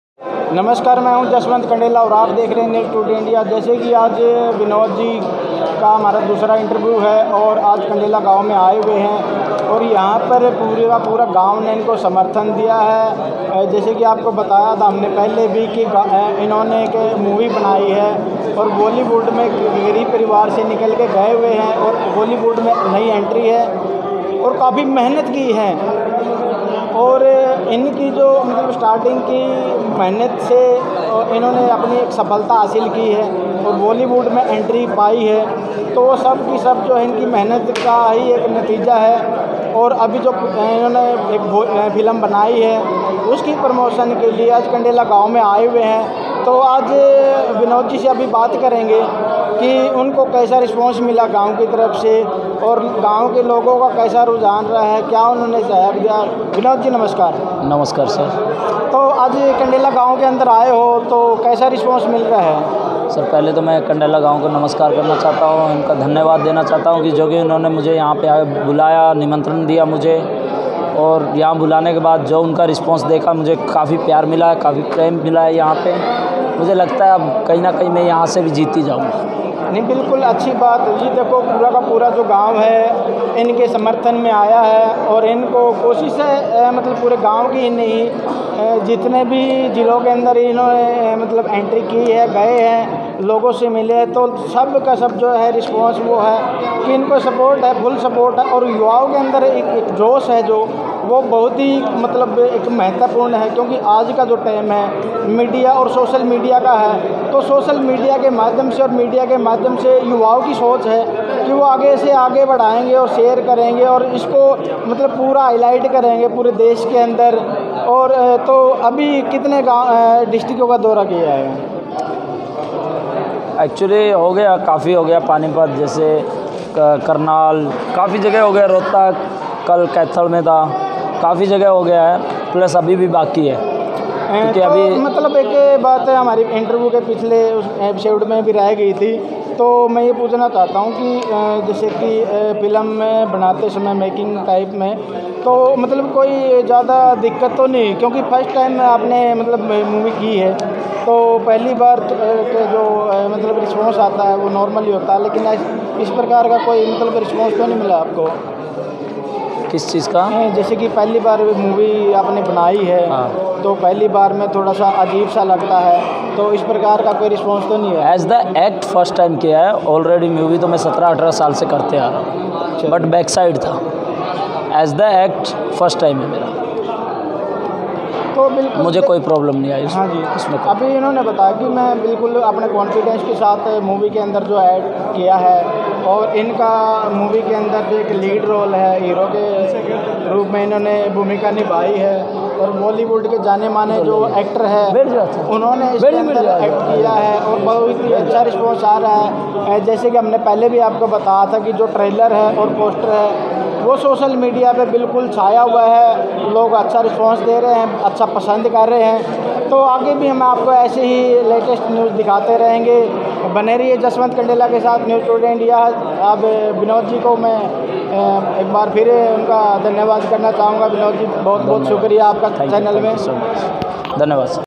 फिल्म(अब ये galtiyan ना हो) की प्रमोशन के लिये कंडेला गांव में पहुंचे अभिनेता vinod dulganch
vinod dulgach
pc24news

ab ye galtiyan na ho |movie by |vinod dulganch,| gauri wankhede released 25 march
about this:-

अपनी पहली हिंदी फिल्म  की प्रमोशन को लेकर कंडेला गाँव मे पहुंचे अभिनेता विनोद दुलगच का गांव कंडेला में हुआ फूल मालाओं के साथ जोरदार स्वागत ,गांव बना खुशी का माहौल  ,गांव के बुजुर्गों ने दिया आशीर्वाद
एक गरीब परिवार से निकल कर किया गांव व हरियाणा का नाम रोशन
गांव की चौपाल में लगी विनोद दुलगच को देखने के लिए भीड़  , गांव के लोगो ने कहा ये फ़िल्म होगी सुपर डुपर हिट
इस मौके पर ,:- सरपंच अजमेर, चेयरमैन टेकराम कंडेला,
समाजसेवी रामकेश रेढू, युवा मंडल के प्रधान बिट्टू कंडेला, सुरेश  फ़ौजी, रमेश, राजेश, पूर्व सरपंच दिलीप भरद्वाज, व गांव के सैंकड़ों लोग शामिल थे